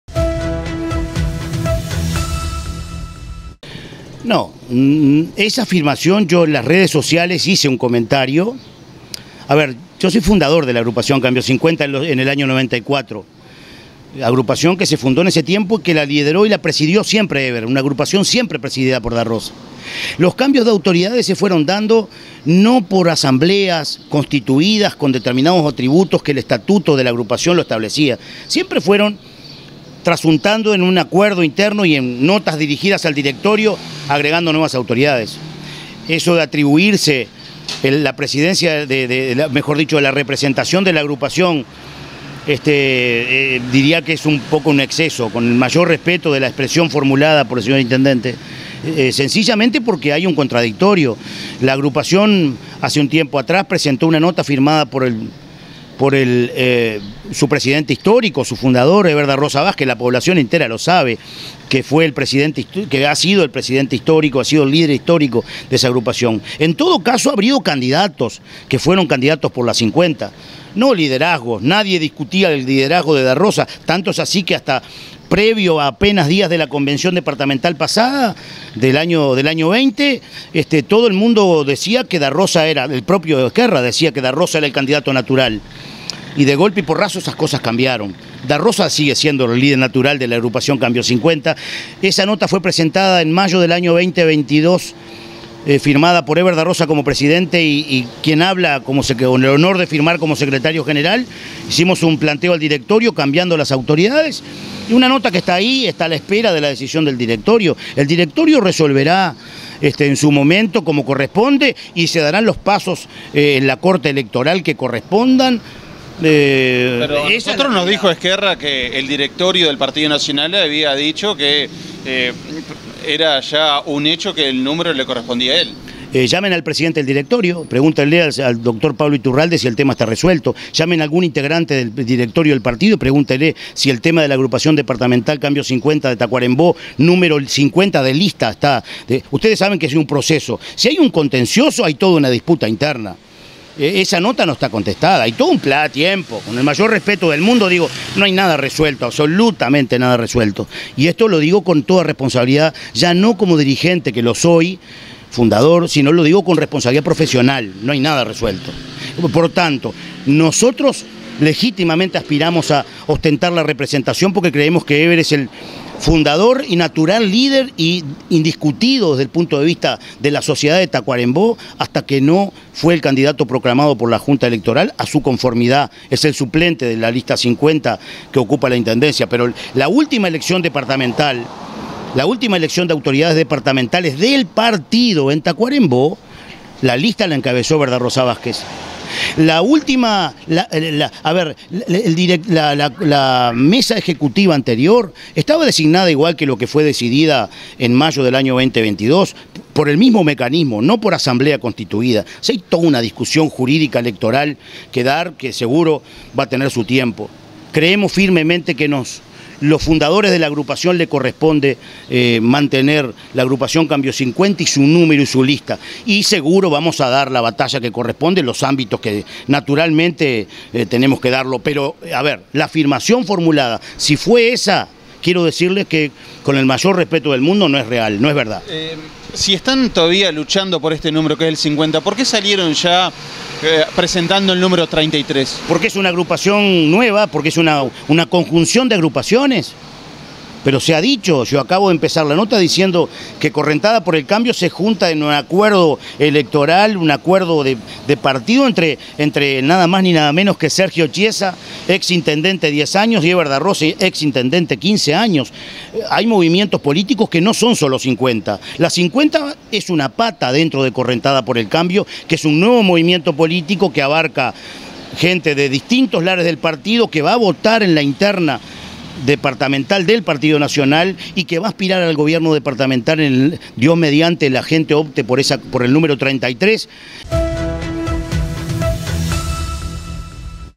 "Estamos peleando por el número 50", dijo Gustavo Martinez
"Eber Da Rosa es el líder histórico de Cambio 50, no está nada definido" comentó el integrante de Correntada por el Cambio